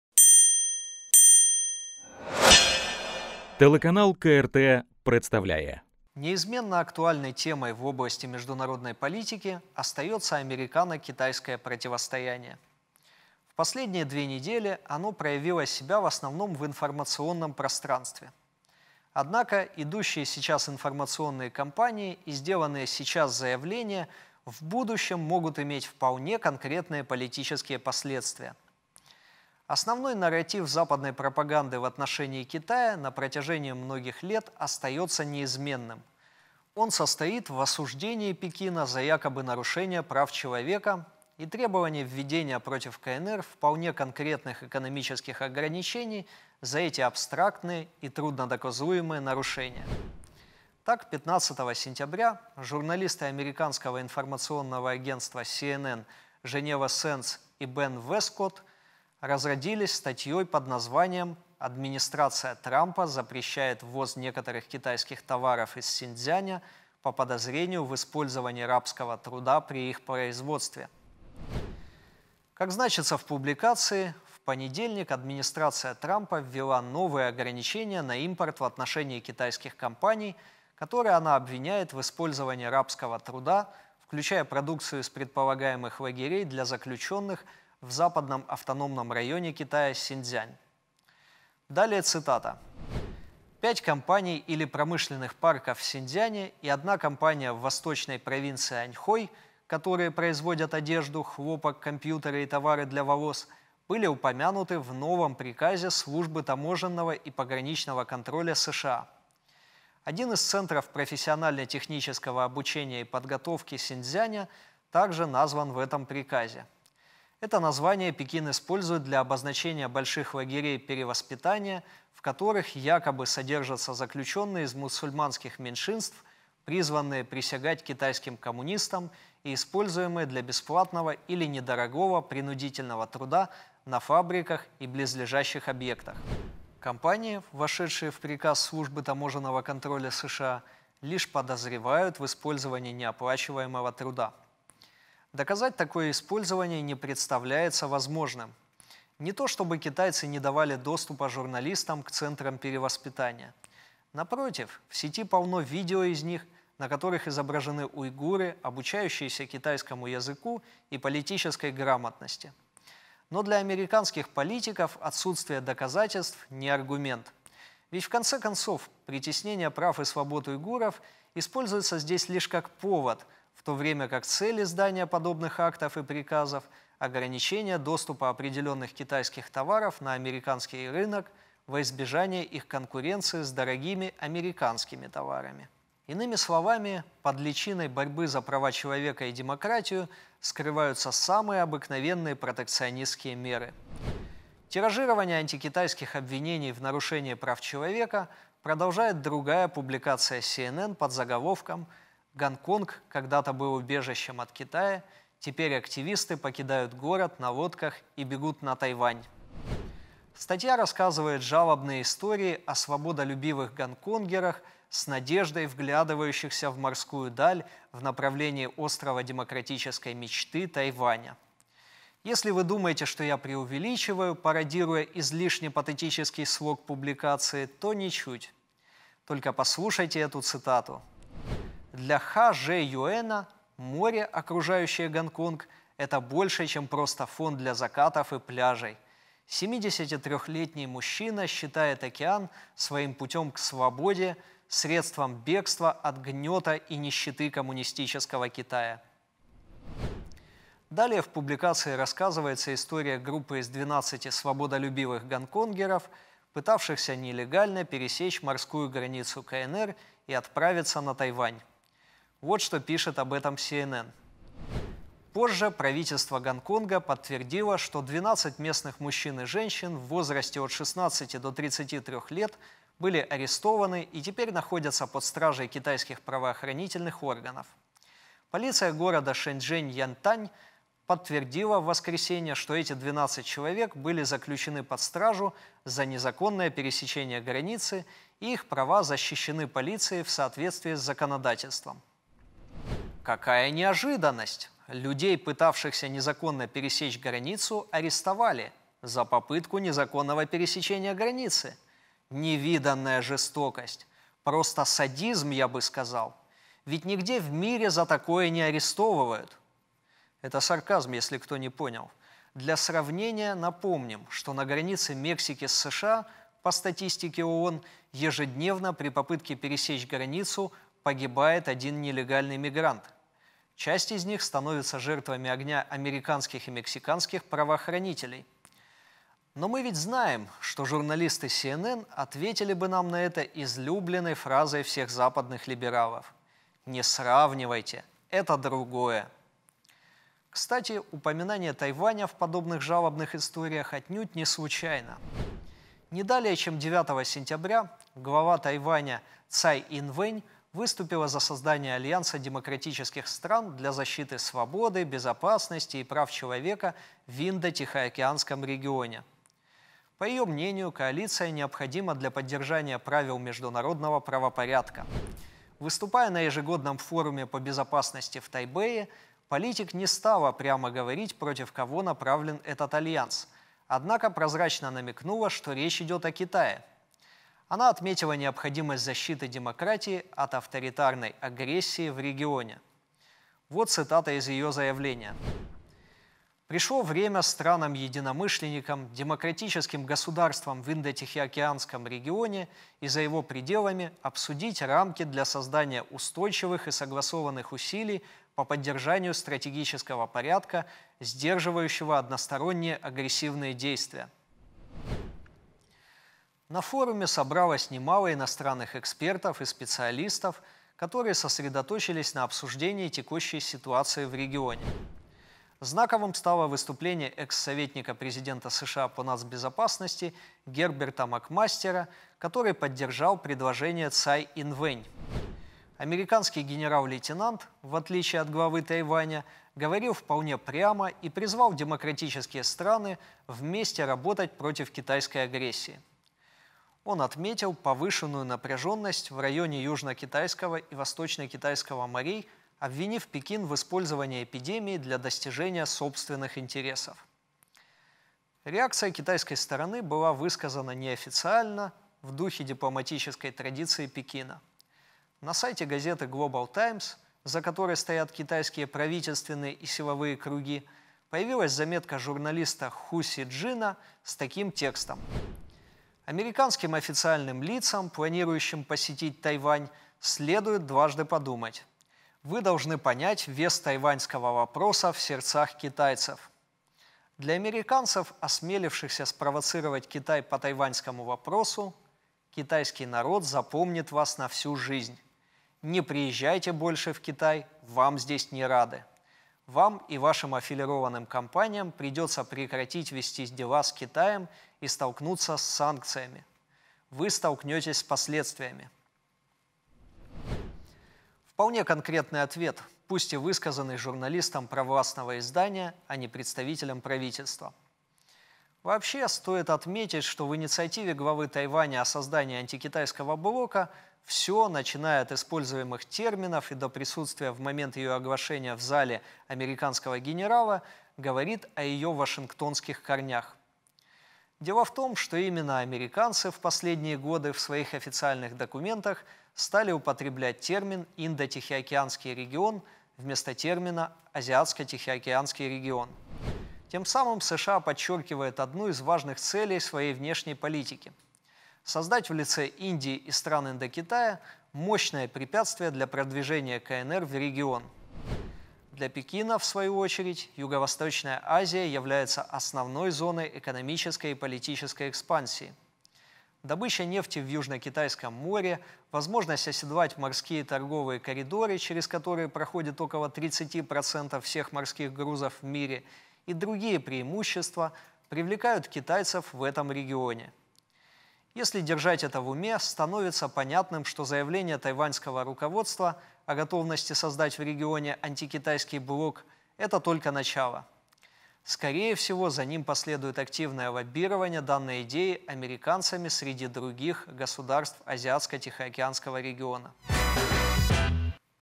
Америка vs Китай: “борьба за демократию” или собственные интересы?
Даниил Богатырёв о кризисе США - Китай